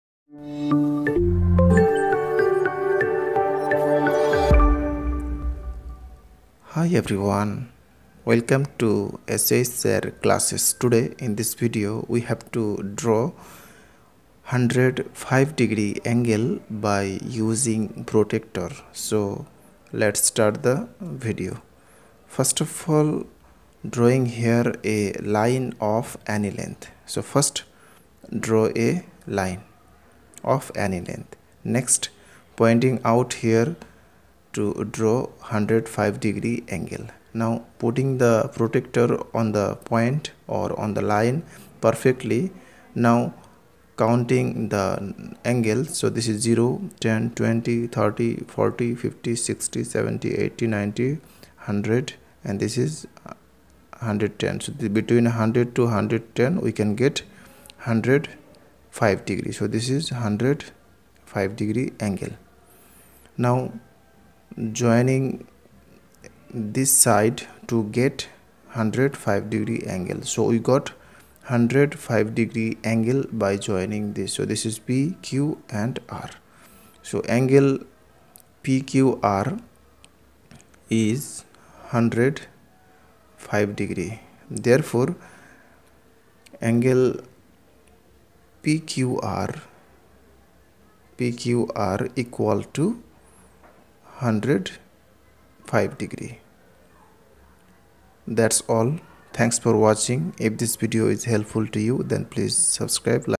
How to draw 105 degree angle using protractor. Make 105 degree angle with protractor. shsirclasses
In Easy Way how to draw 105 degree angle using protractor. make 105 degree angle with protractor.